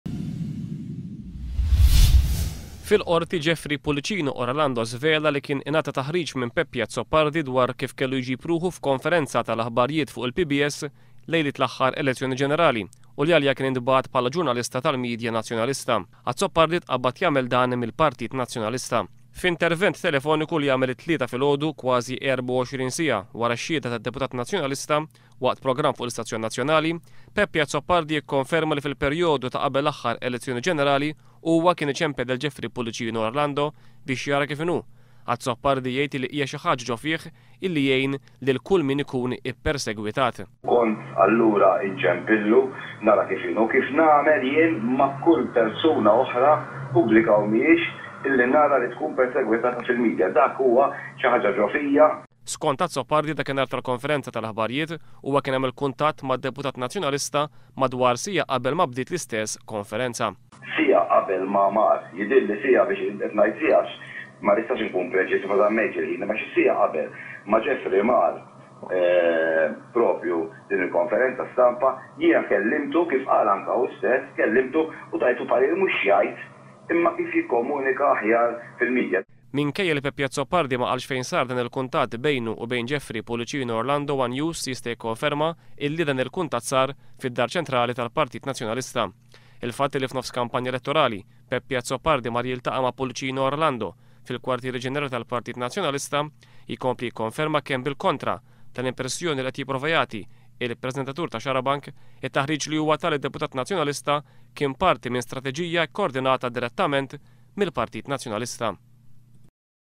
Peppi Azzopardi jikkonferma li ħarreġ lil Jeffrey Pullicino Orlando .
Peppi Azzopardi, il-prezentatur tal-programm Xarabank fuq l-istazzjon nazzjonali, jikkonferma dak li qal fil-qorti d-deputat Nazzjonalista Jeffrey Pullicino Orlando dwar it-tahrig li huwa ta lill-istess deputat Nazzjonalista lejliet l-ahhar elezzjoni generali.Azzopardi jghid li huwa dejjem jghin lil kull min fi kliemu jkun ippersegwitat fil-media, ghaliex dik hija xi haga go fih .